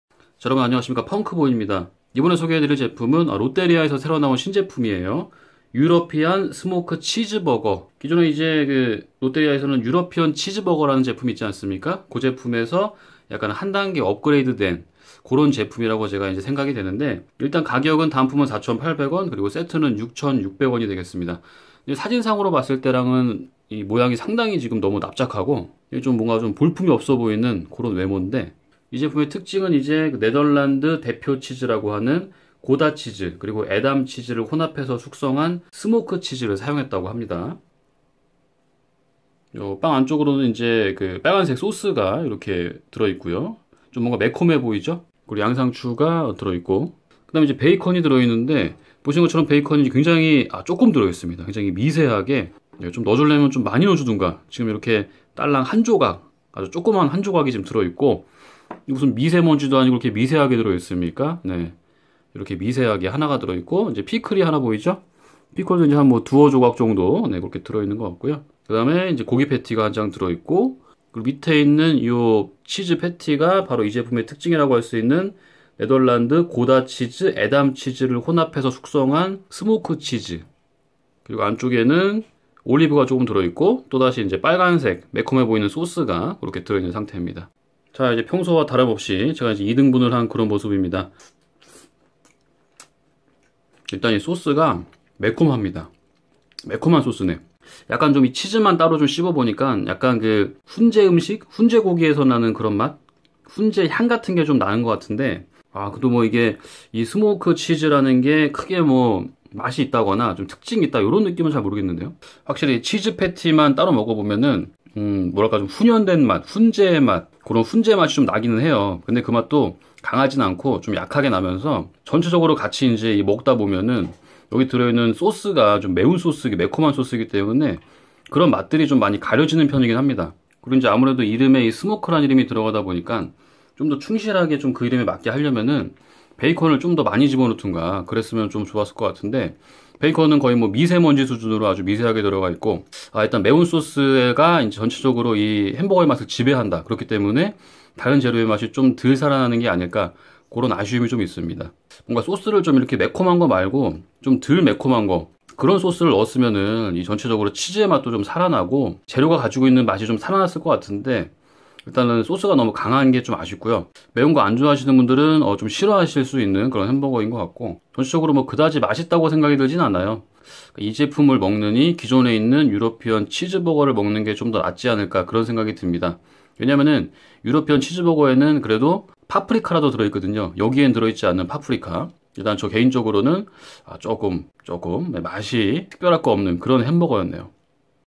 [롯데리아 신메뉴 유러피언스모크치즈버거] 롯데리아 신메뉴를 리뷰하여보자 LOTTERIA European Smoked Cheese Burger
롯데리아 신메뉴 유러피안 스모크 치즈버거를 먹어보았습니다!!
흠...유러피안치즈버거의 업그레이드 버전이라고 내놓은 모양인데..
매콤한 맛빼곤 별다른 맛이 없는 것 같군요..ㄷㄷㄷㄷ